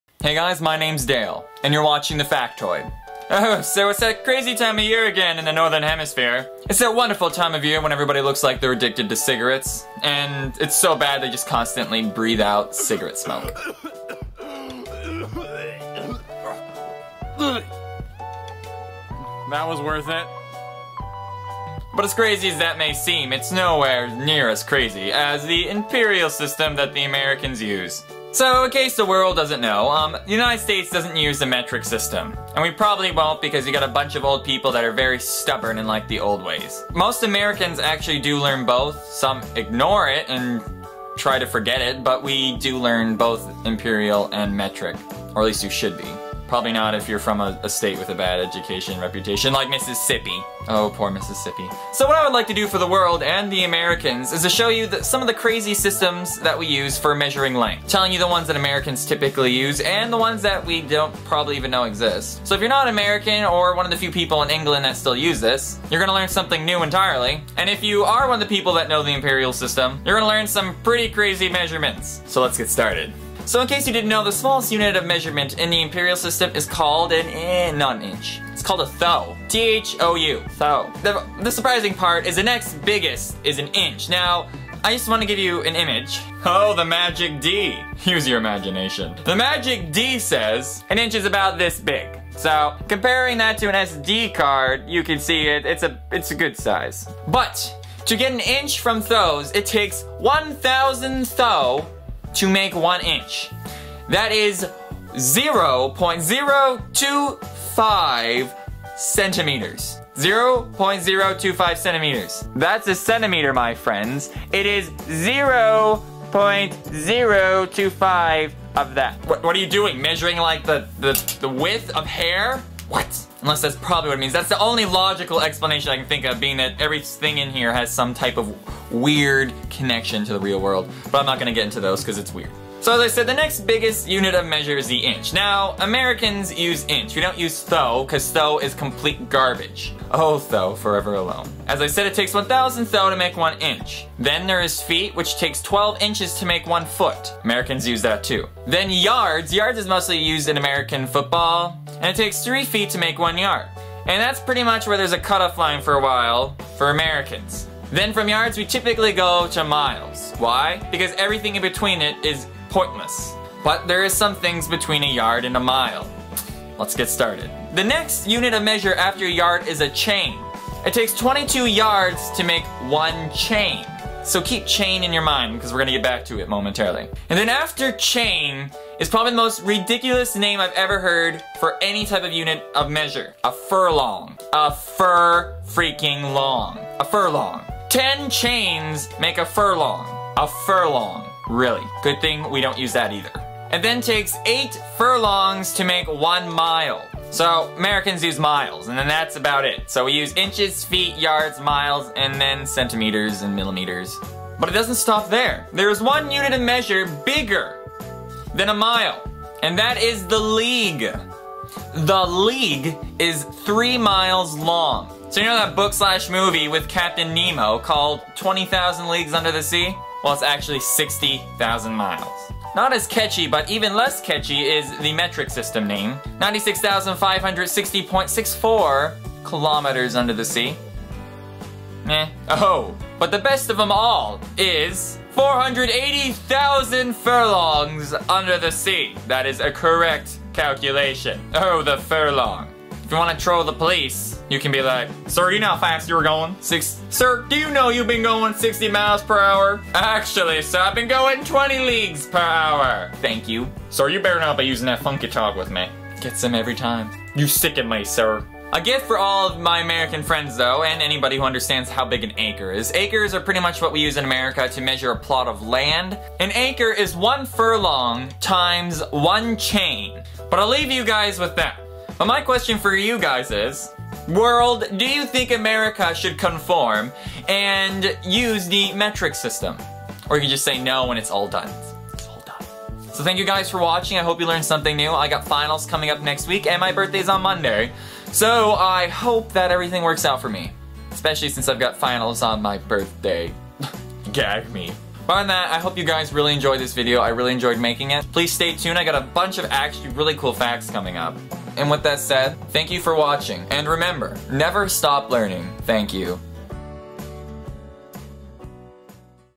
The US's Crazy Measuring System. Time for Metric?
This might be a good reason for us to start considering moving away from the imperial system.  Mostly because it's crazy.  Let me know what you think and I hope you enjoy the video.

It should be noted that the U.K. had some other forms and names for measurements smaller then an inch, but being that the U.S. is the only nation that uses this Imperial system, I only added what the U.S. recognizes.